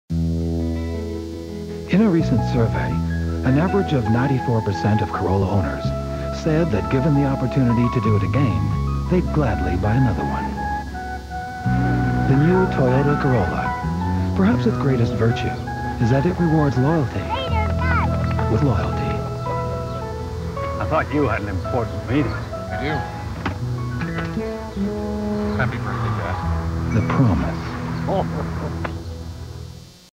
1991 Toyota Corrola Commercial
Ad for the 1991 Toyota Corrola - Son buys golf clubs for his dad.